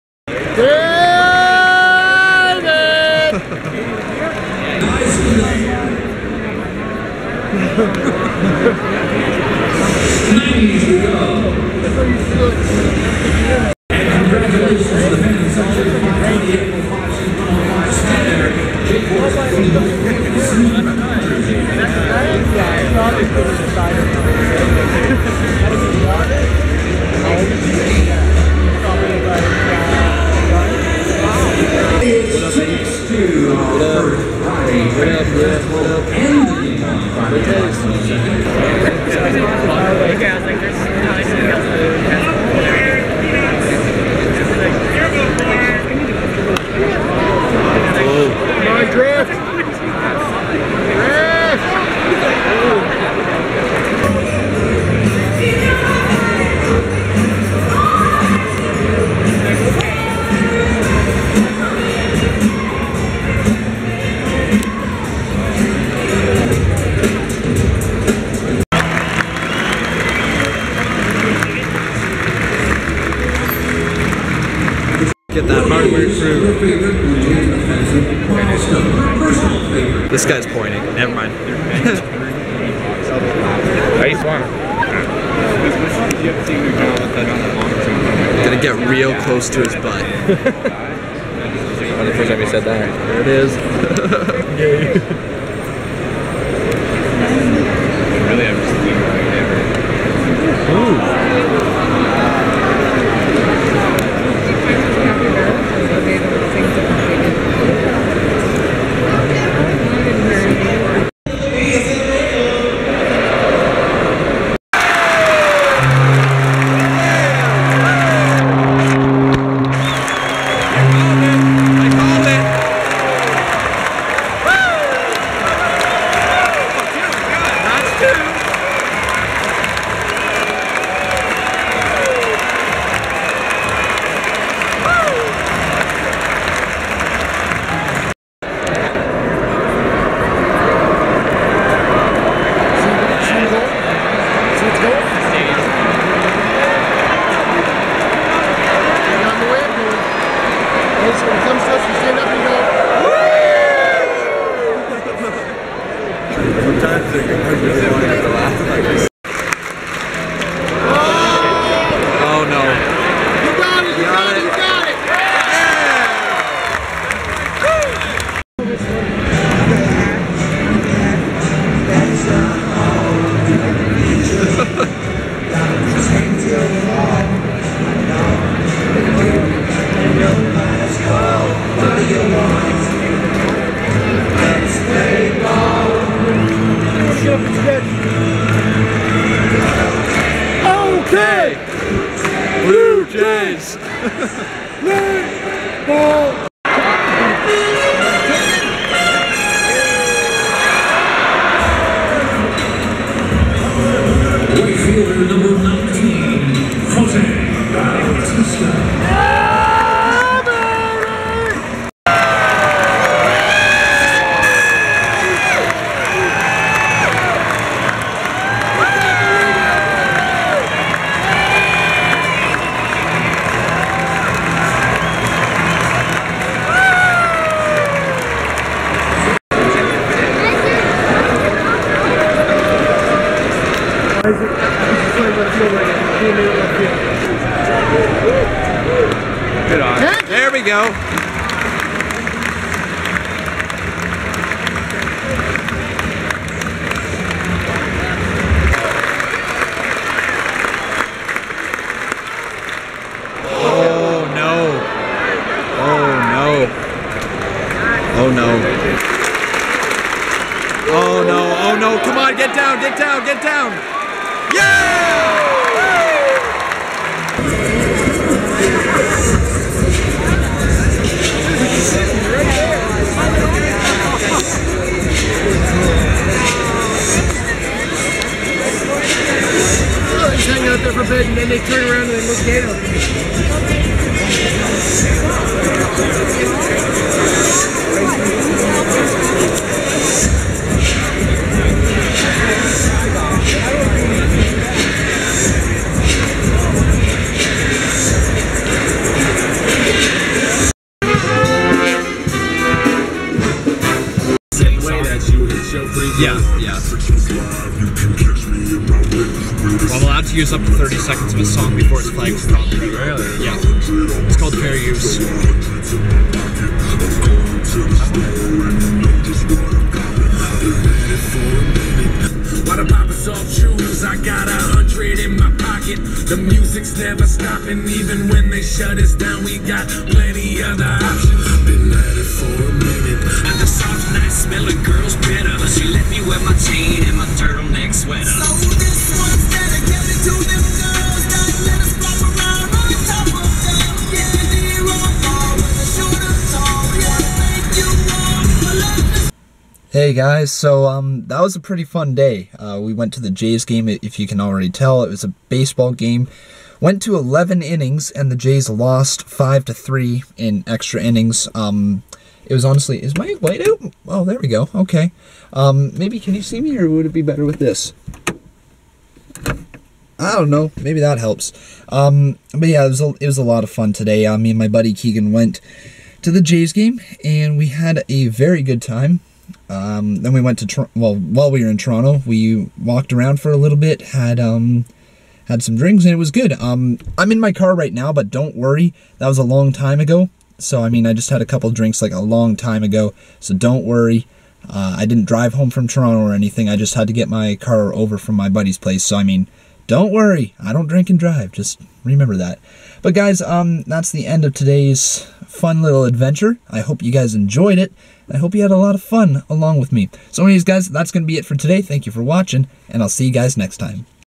Toronto Blue Jays vs Boston Red Socks Live Vlog
So today I went to the toronto blue jays baseball game where they lost 5-3 against the Boston Red Socks in 11 innings.